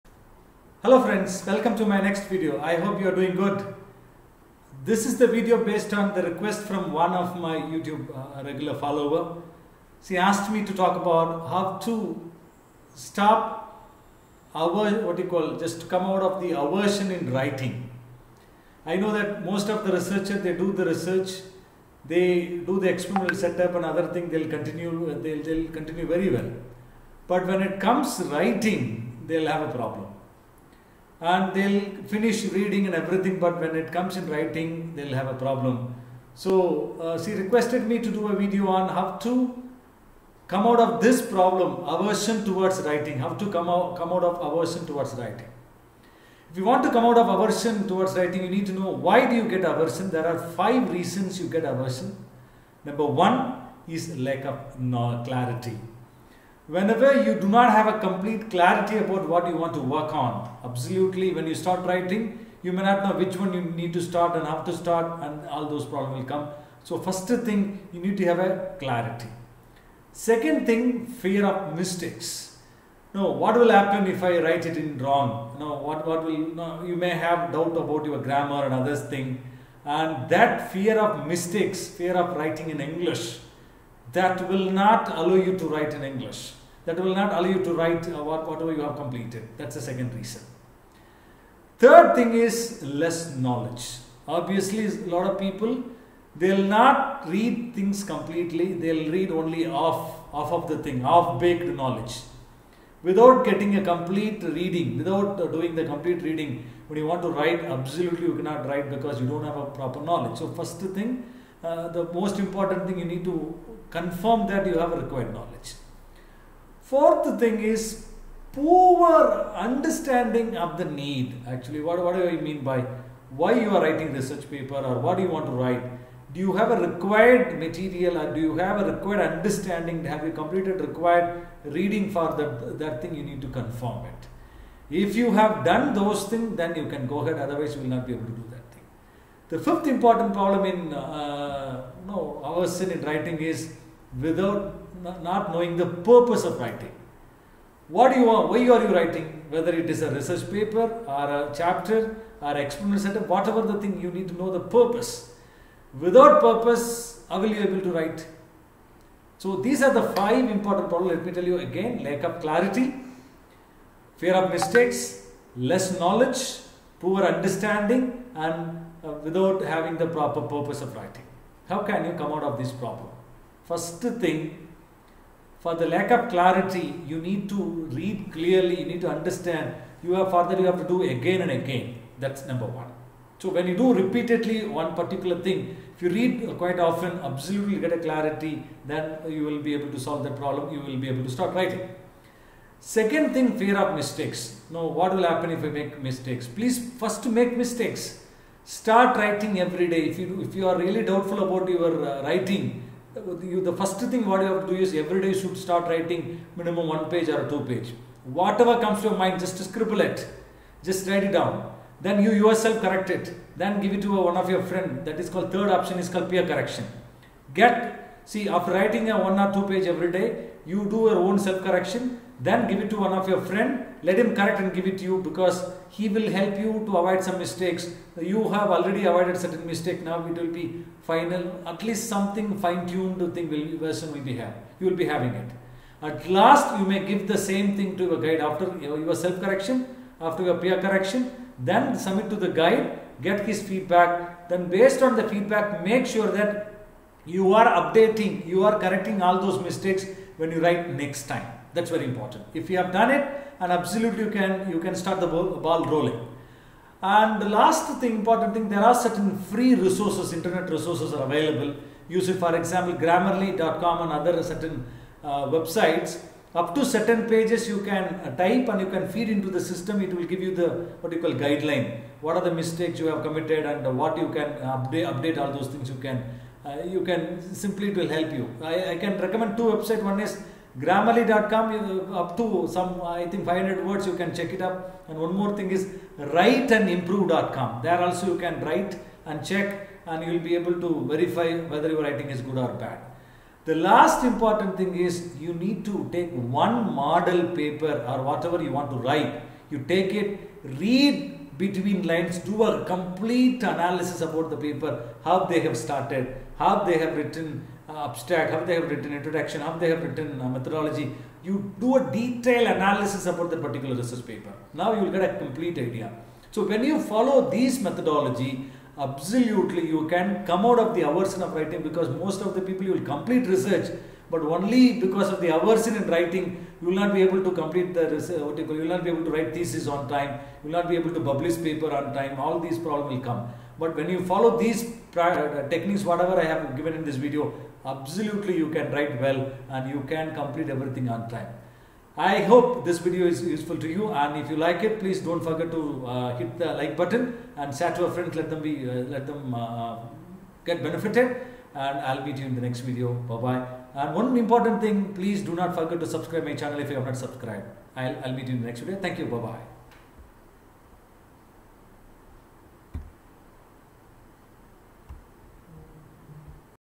Tips to avoid Writing Apprehension | Research
Writing is always a big problem to many of the researchers and they become clueless after completing the research on converting into a document. How to start? Where to start? There are big apprehensions always prevents them from writing. This video will definitely guide you to come out of your writing with simple tips to overcome aversion of writing..

Links of my other research videos